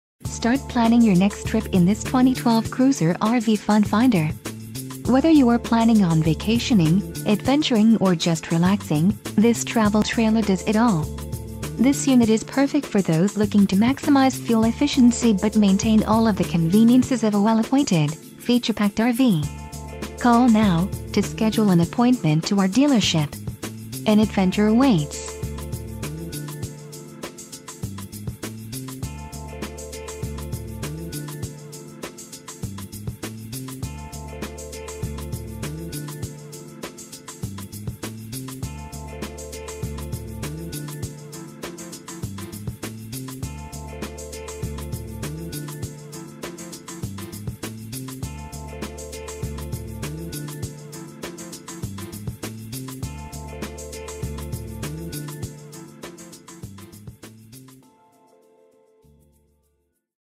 2012 Cruiser RV Fun Finder 210 UDS - George M Sutton RV -...
Now at Sutton RV in Eugene Oregon. The Lightweight, great looking and full featured describes this 2012 Funfinder 210 UDS travel trailer. The perfect RV for today&apos;s camping family. Interior U-dinette with comfort corners Neo-angle glass shower door Stainless-look refrigerator front Stianless steel sink with gooseneck faucet 2-sink covers Large storage cabinets Directional decorative lighting Roller bearing drawer guides Fabric box valances with curtains Pleated night shades Bedspread Double pocket screwed cabinets Beau-flor cold crack resistant flooring 5 in. high density cushions Hidden hinges on cabinet doors Laminate countertops with matching edge Handmade medicine cabinet with mirror Luxury residential style furniture Overhead front cabinets Skylight and fan in bathroom Walkaround bed Exterior / ConstructionLaminated, crowned 3 1/2 in. roof with foam brick insulation Flush floor total control slideout R... read more